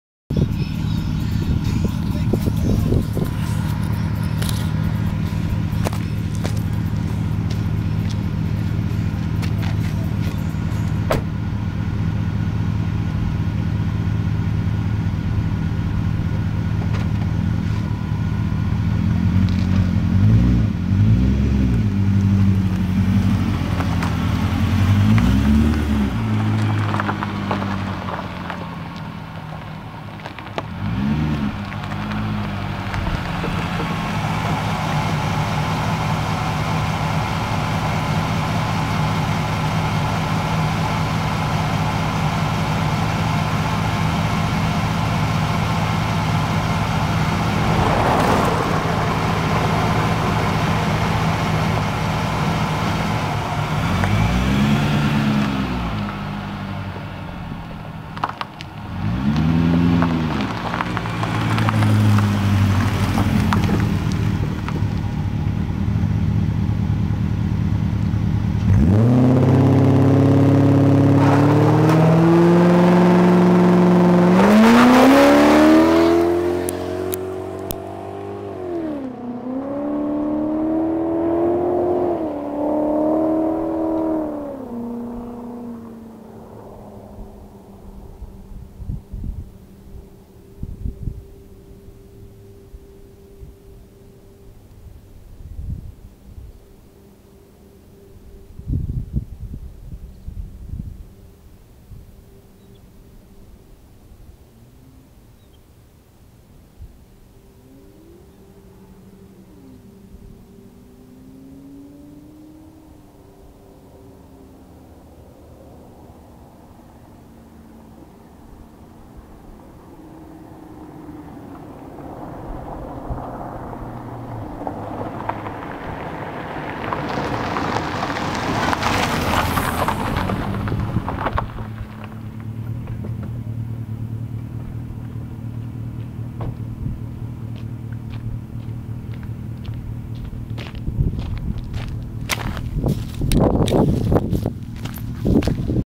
Dynomax, (first road clip)
Fresh off a 760I dual fuel pump install and seasoning of muffler fibers from a few hundred miles of driving.
Country road has a some loose gravel on pavement so I backed off after about 150 ft because it would not hook up.  BTW, little speakers don't do this clip any justice, lost my sub and my playback is nowhere close to the real sound.